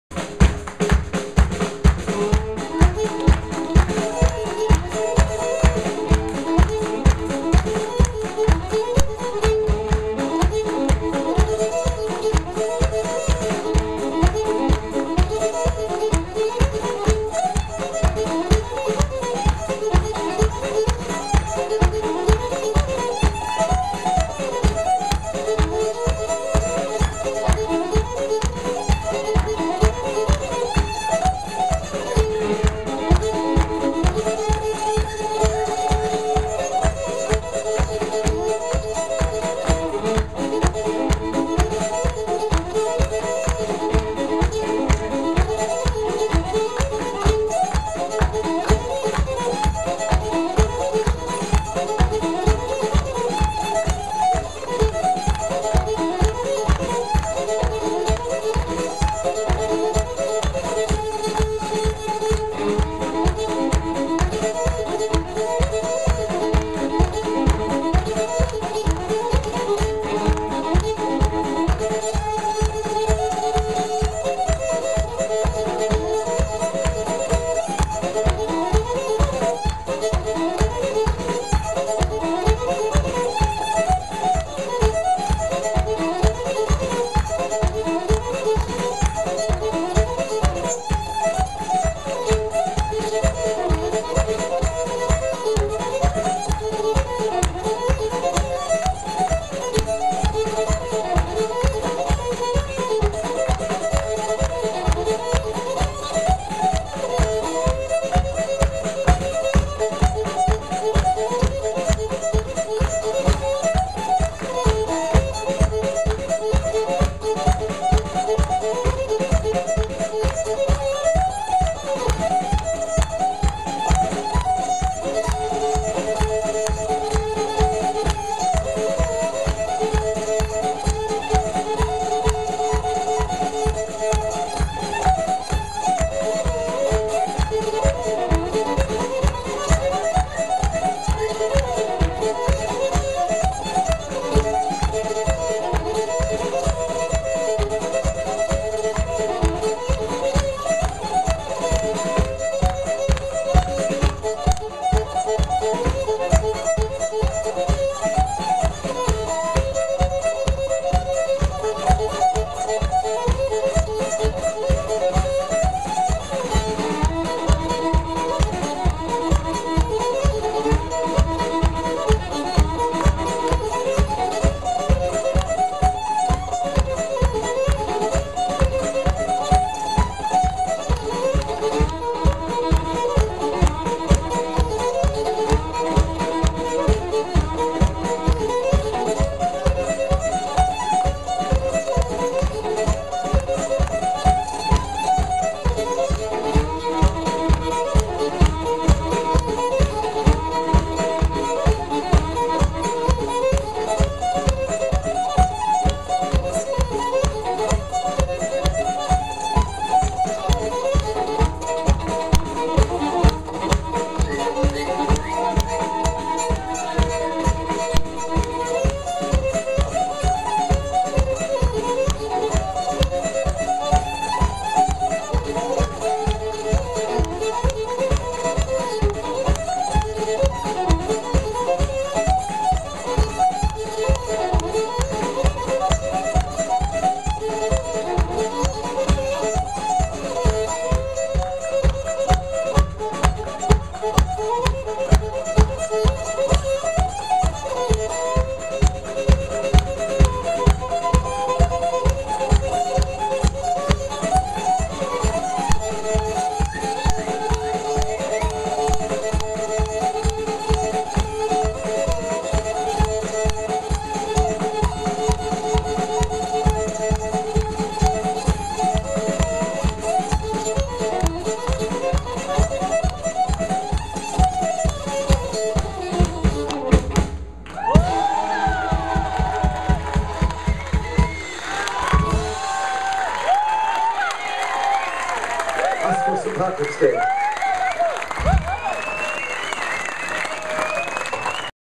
StPatricksDaySpecial performed by Pat McManus & Paul Faloon @ Spirit of 66,Verviers,Belgium, 16.3.12
Extraordinary gig by those excellent musicians.